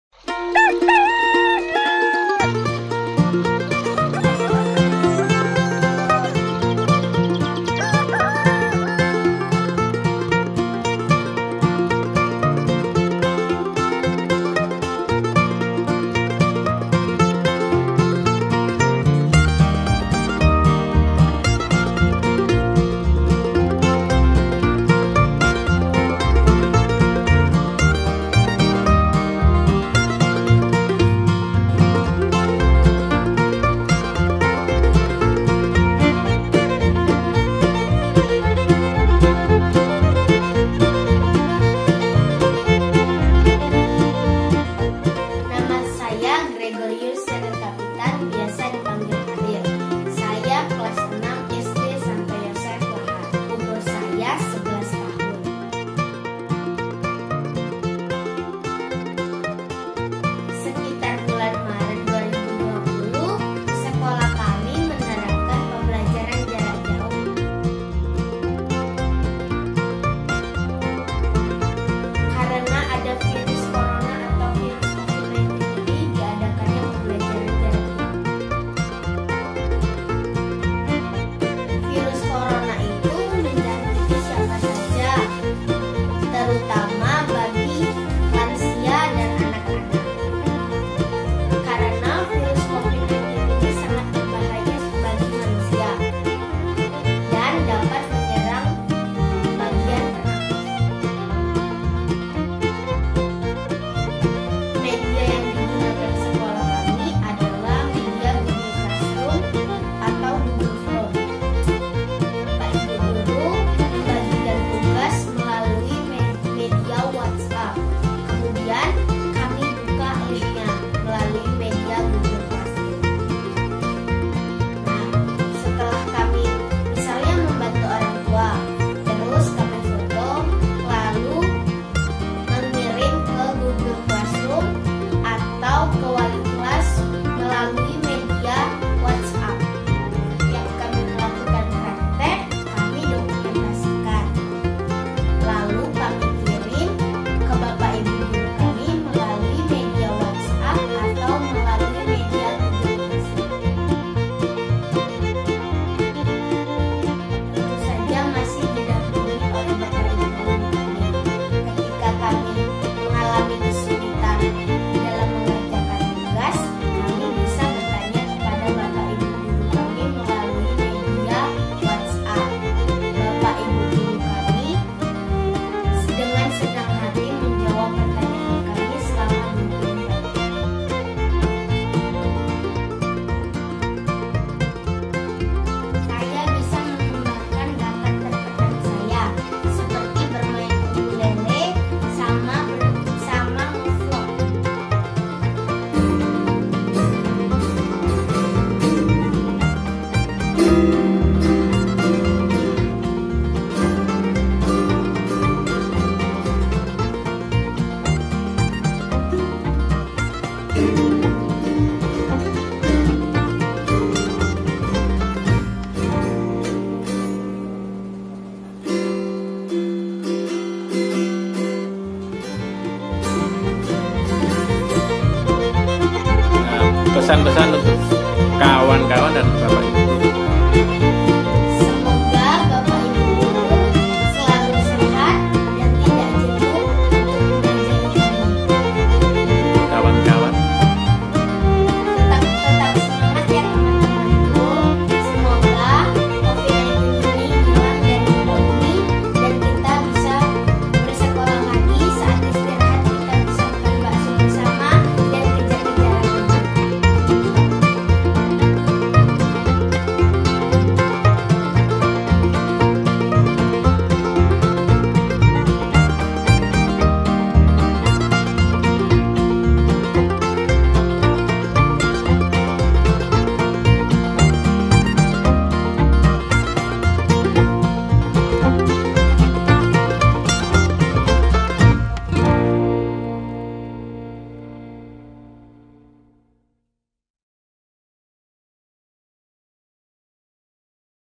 SEMANGAT PAGI MENGGAPAI MIMPI DI MASA PANDEMI COVID 19
Cerita Pengalaman Belajar Di Masa Covid-19